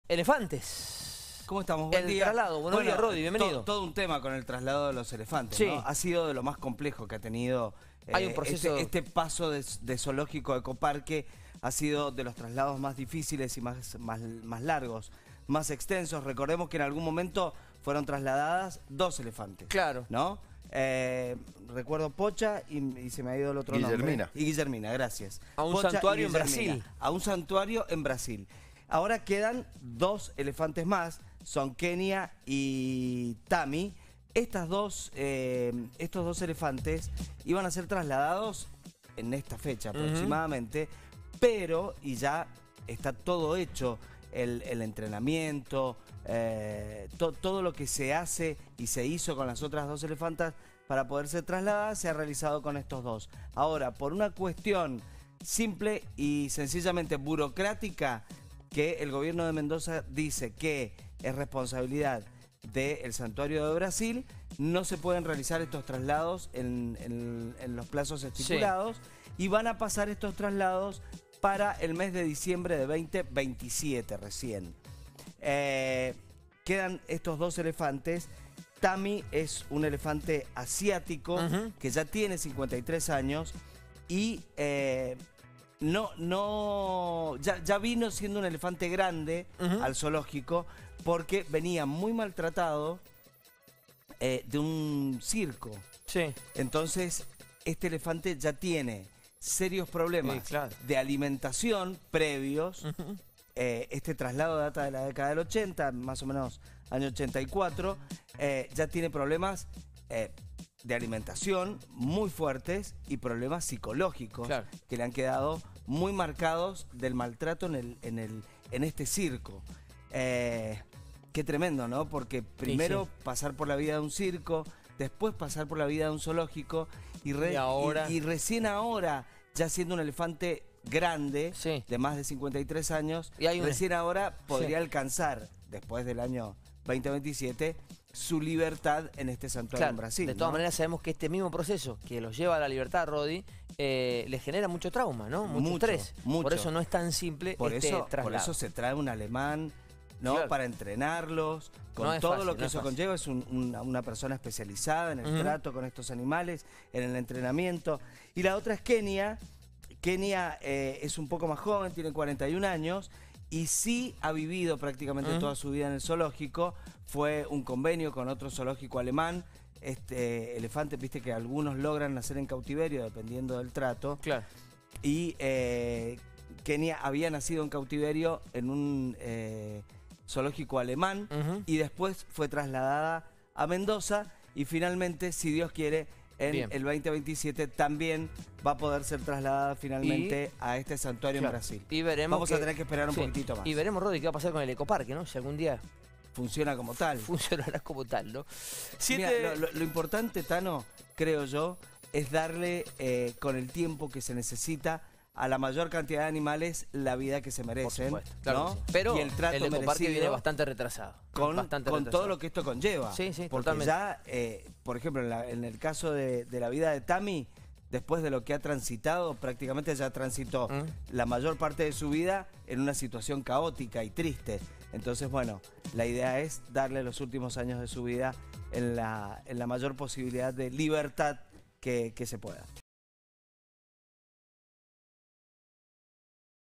El plazo del traslado será hasta el 31 de diciembre del 2027
Las autoridades provinciales en Mendoza acordaron prorrogar hasta el 31 de diciembre de 2027 el traslado de los elefantes Kenya y Tamy desde el ex Zoológico provincial a un Santuario de Elefantes en Brasil.